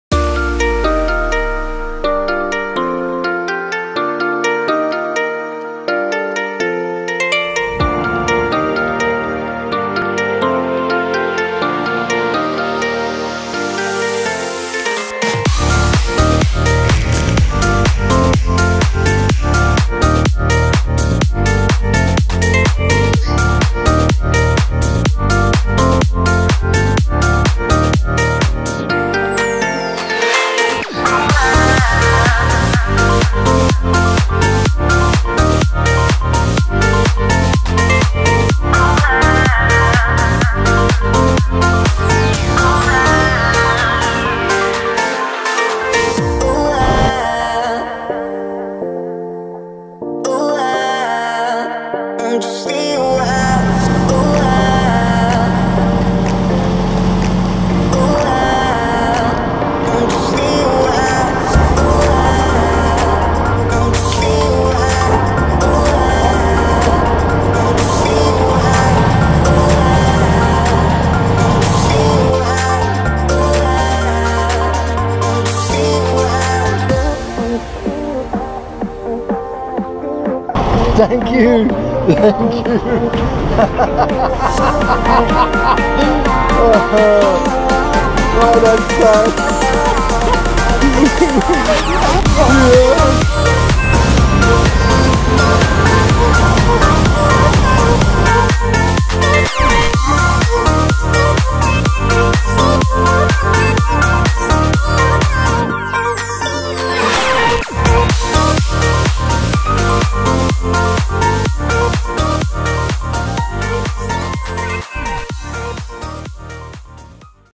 Bangkok Water Taxi's
Bangkok Canal - water taxi's: Schedules, locations, tickets, how to pay, costs and getting on and off, not for the faint hearted, they're the fastest in the world!